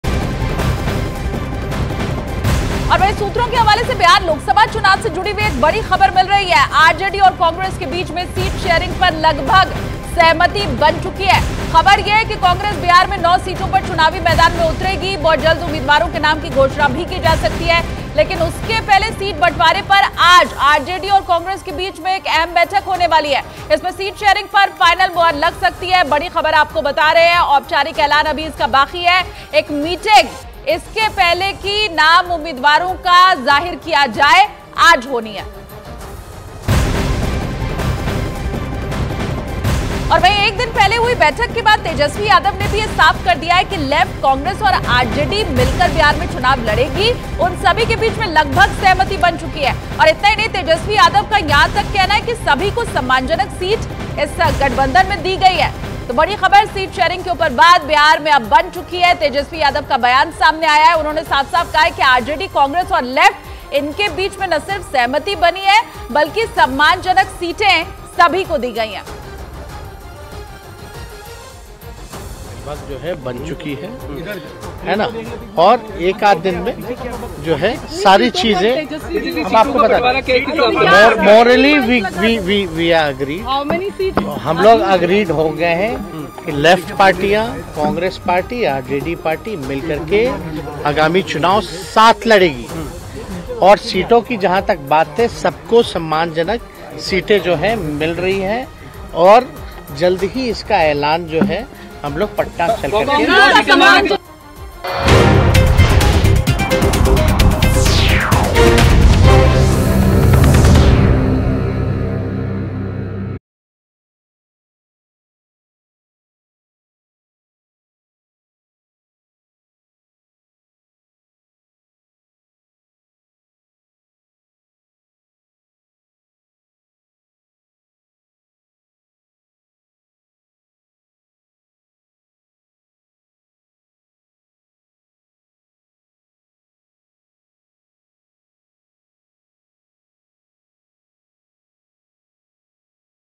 Election Breaking : RJD और कांग्रेस सीट शेयरिंग पर लगभग सहमत! | Congress | Tejashwi Yadav | Bihar
सूत्रों के हवाले से बिहार लोक सभा चुनाव से जुड़ी एक बड़ी ख़बर आ रही है। RJD और कांग्रेस के बीच सीट शेयरिंग पर लगभग सहमति बन गई है। और ख़बर है कि कांग्रेस बिहार में 9 सीटों पर चुनावी मैदान में उतरेगी। और जल्द ही उम्मीदवारों के नाम की घोषणा भी हो सकती है। लेकिन उससे पहले सीट बंटवारे पर आज RJD और कांग्रेस के बीच एक अहम बैठक होगी। जिसमें सीट शेयरिंग पर फ़ाइनल मुहर लग सकती है।